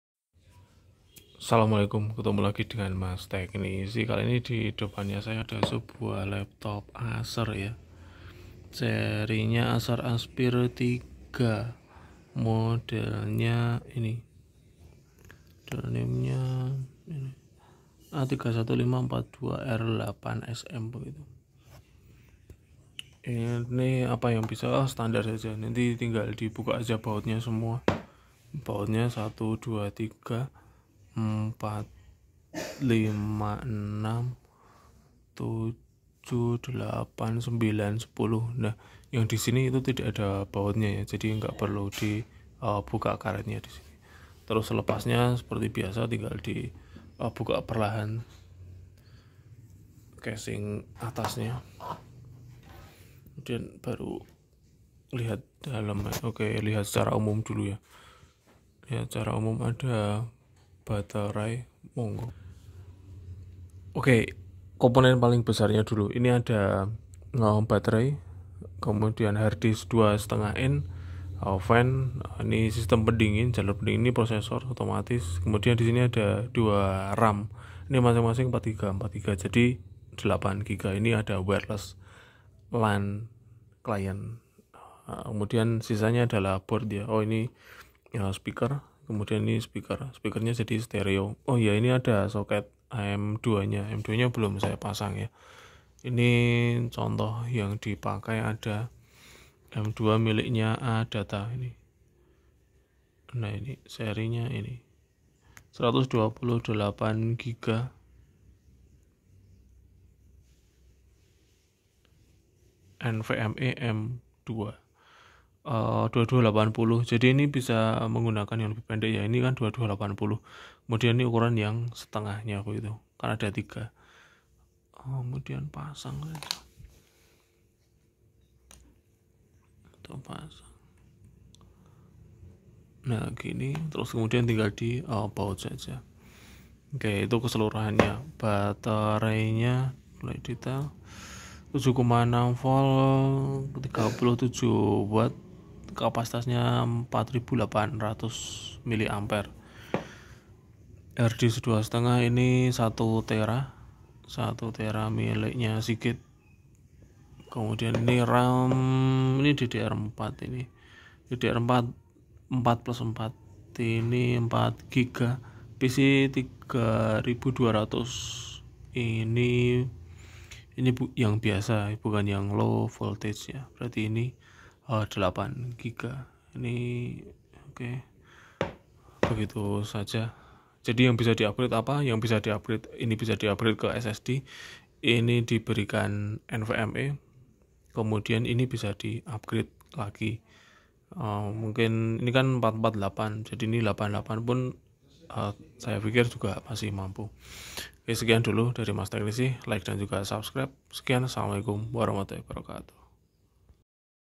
Apa yang bisa diupgrade di Laptop Acer Apire 3 Acer Aspire 3 A315-42
AMD Athlon II (300U, 2.40 GHz, 4 MB) -
39.6 cm (15.6") LED -
16:9 Full HD -
LCD - ComfyView -
AMD Radeon™ Vega 3 -
8 GB DDR4 SDRAM (sudah upgraded +4GB)
No - Colour Shale Black -
Weight (Approximate) 1.90 kg